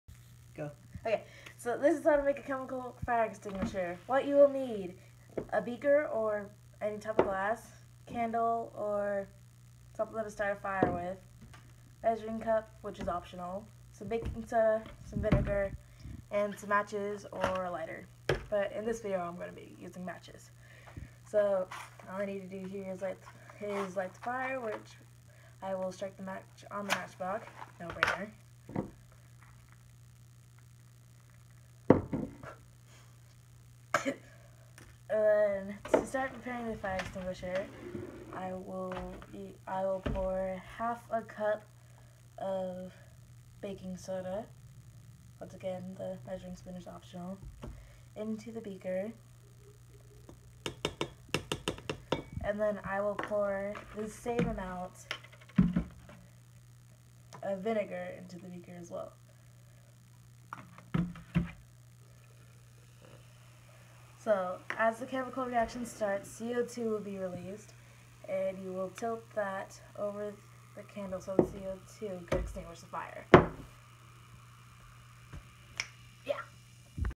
DIY Fire Extinguisher by Cari
Chemical Reactions Fire Extinguisher project made by a student at RVCS-Matanzas